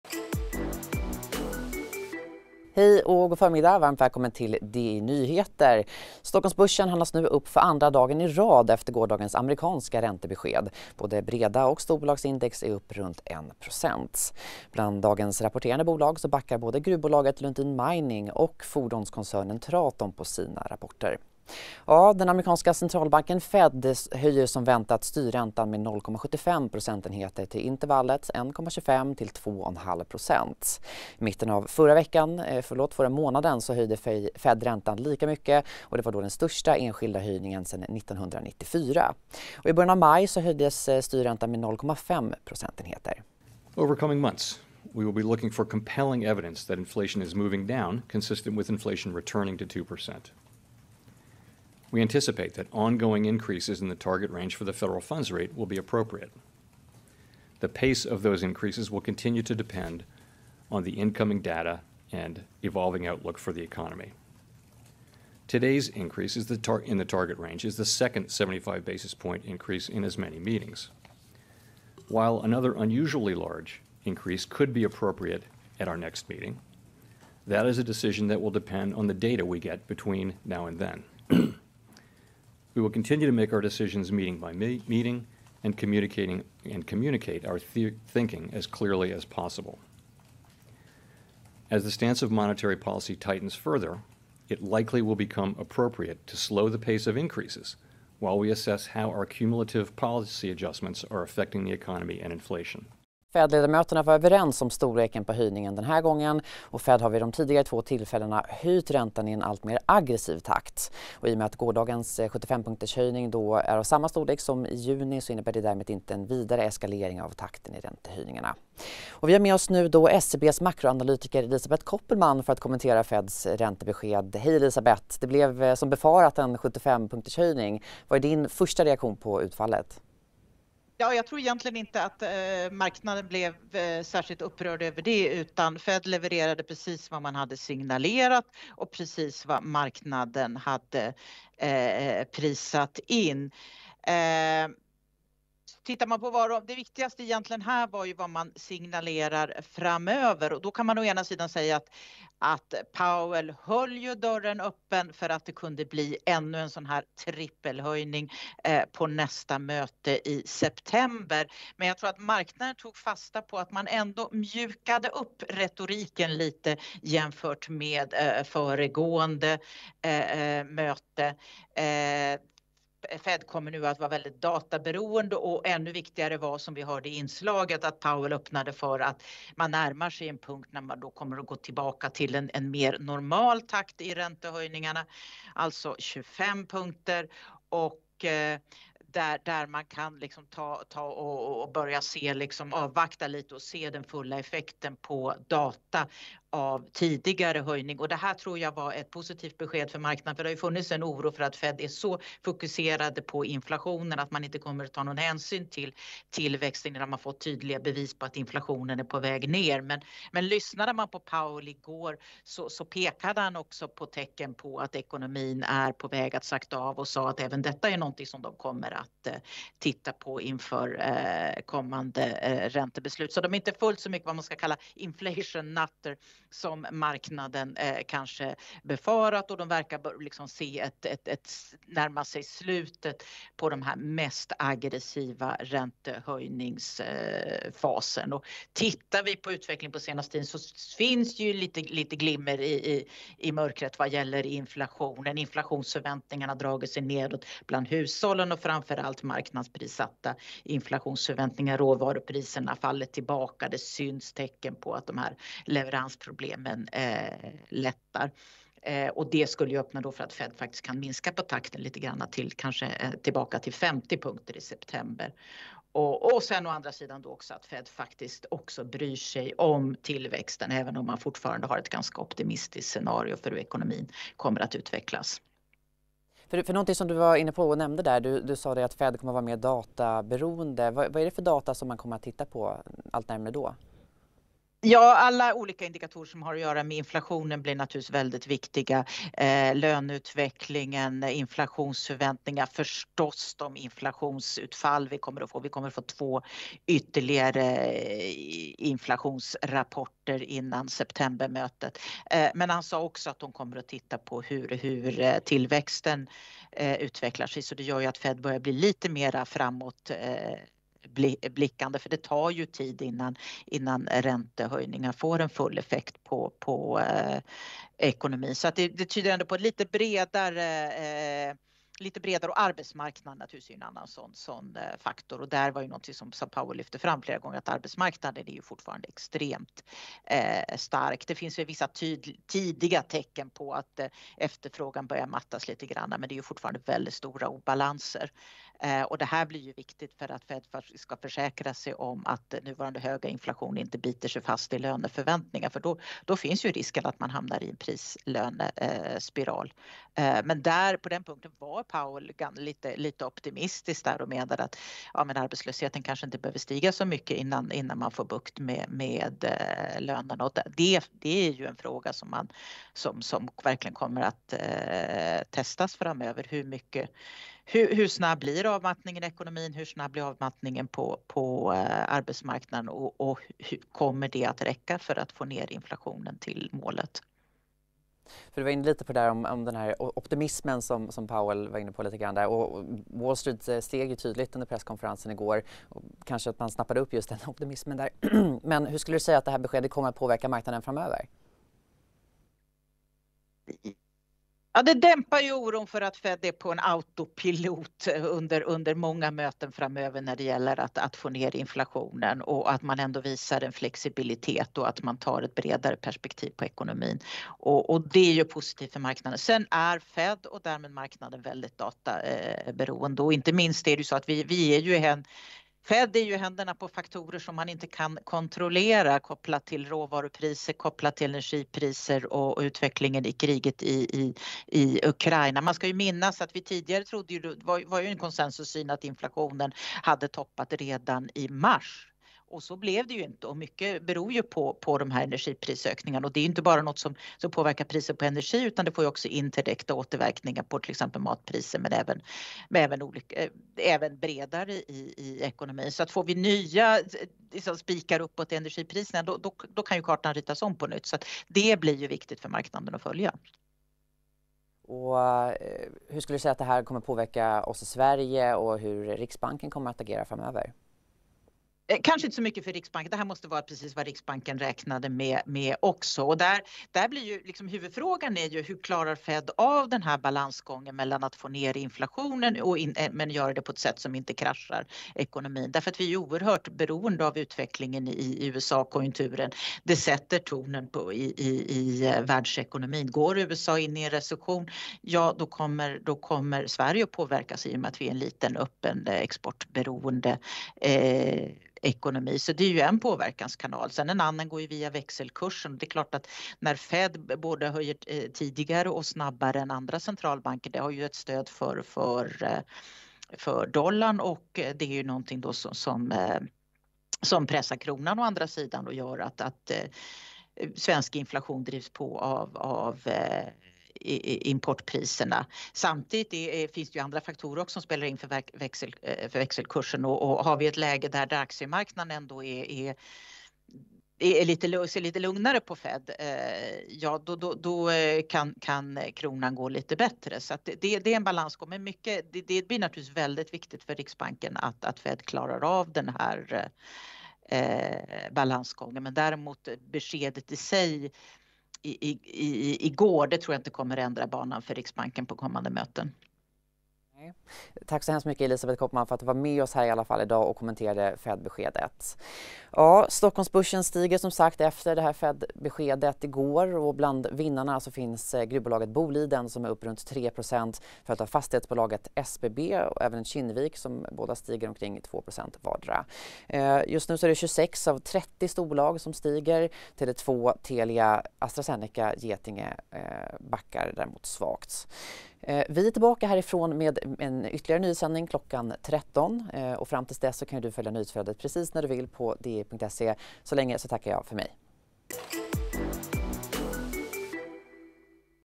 Makroanalytikern: Så påverkas börsen av Feds räntebesked
SEB:s makroanalytiker Elisabet Kopelman kommenterar räntebeskedet i Di Nyheter med Filippa Fernqvist.